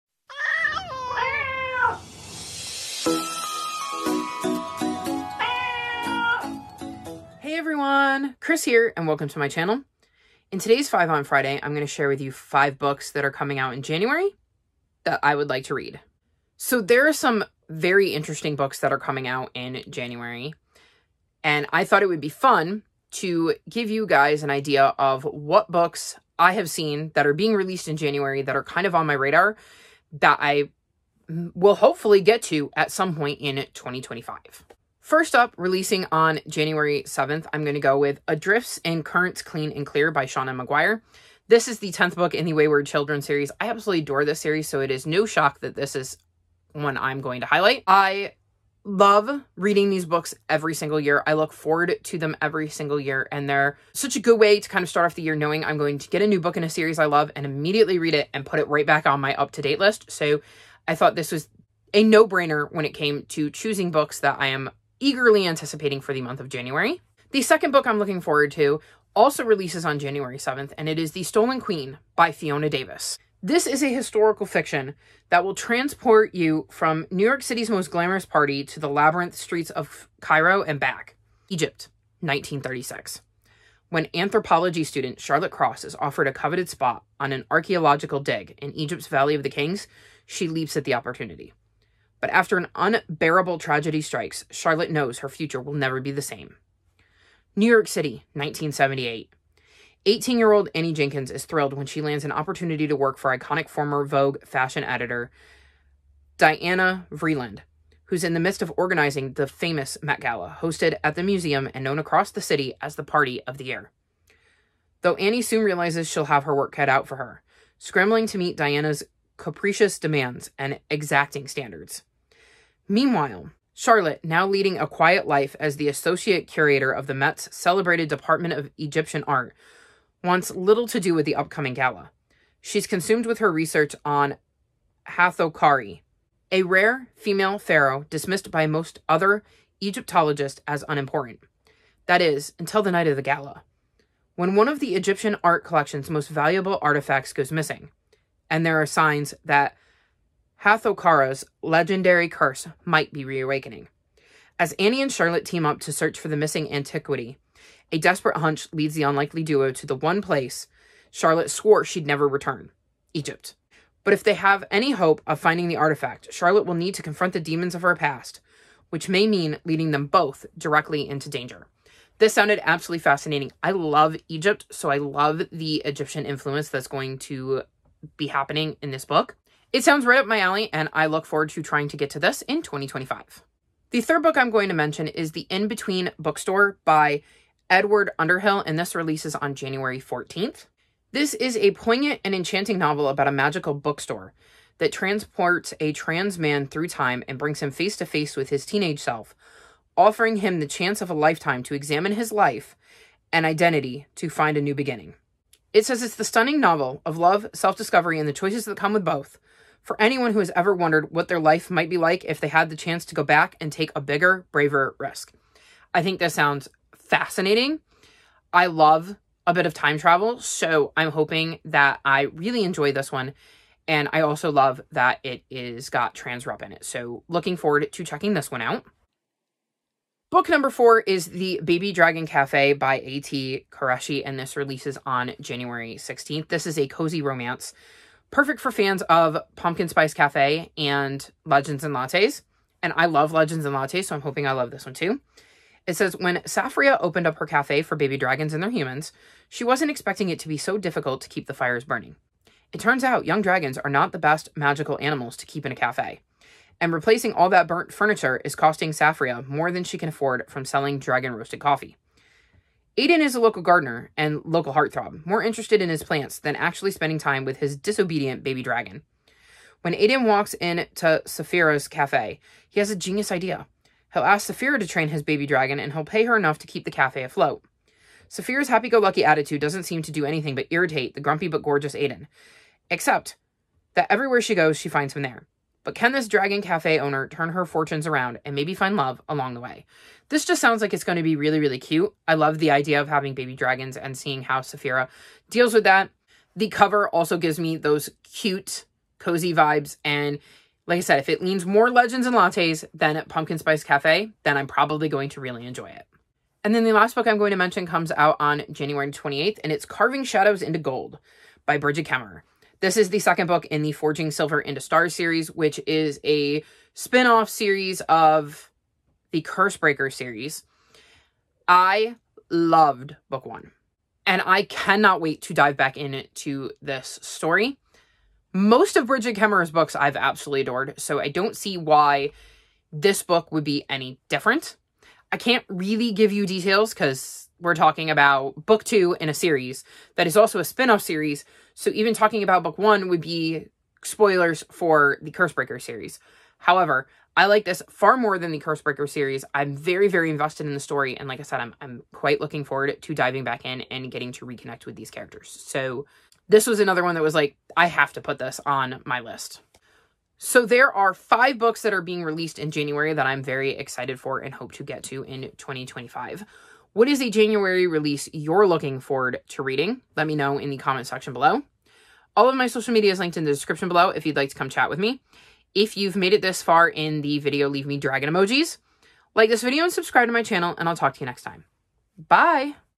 Five on Friday #78 - Anticipated January Releases [CC]
Here are 5 books coming out in January that I am looking forward to reading!